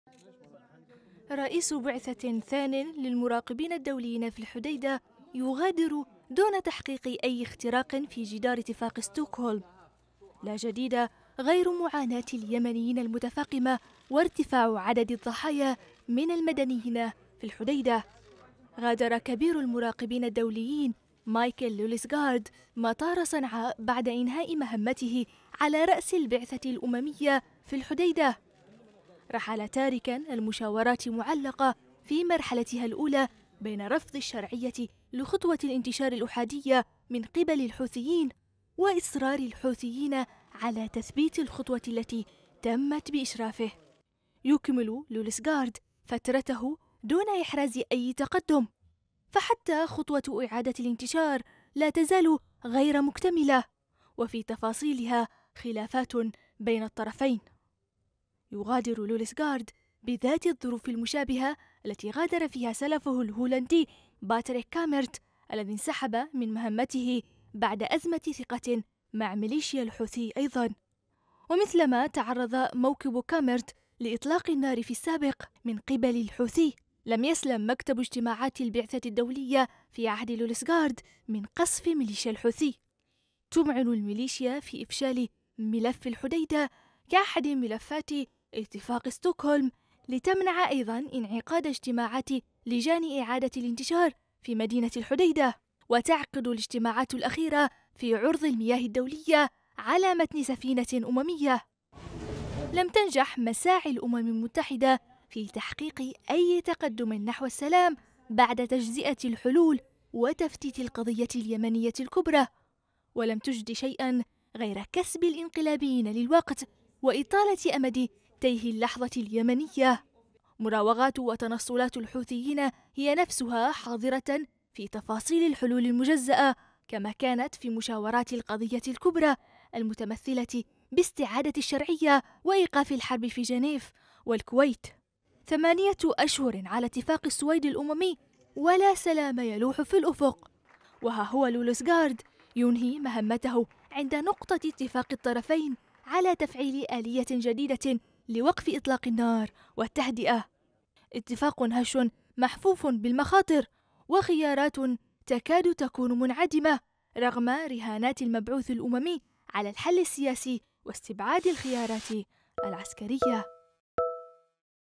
لوليسغارد يغادر اليمن بعد انتهاء مهمته على رأس البعثة الأممية | تقرير: أمين صالح
غادر كبير المراقبين الدوليين مايكل لوليسغارد مطار صنعاء بعد إنهاء مهمته على رأس البعثة الأممية في الحديدة، ونقلت مصادر صحفية عن مسؤولين مطلعين قولهم إن لوليسغارد، طلب إنهاء ولايته بعدما اختارته بلاده لتولي وظيفة عسكرية رفيعة، وكان الدنمركي لوليسغارد قد تولى منصبه نهاية يناير الماضي، خلفا للهولندي باتريك كاميرت، الذي انسحب من مَهمته هو الآخر بعد أزمة ثقة مع مليشيا الحوثي ، ولم تكشف الأمم المتحدة عن الرئيس الجديد لبعثتها في محافظة الحديدة ، وسط استمرار الخروقات والتصعيد العسكري في المدينة ومحيطها، وكانت اللجنة الأممية لتنسيق إعادة الانتشار قد أعلنت التوصل الى اتفاق على تفعيل آلية وتدابير جديدة لتعزيز وقف إطلاق النار والتهدئة ...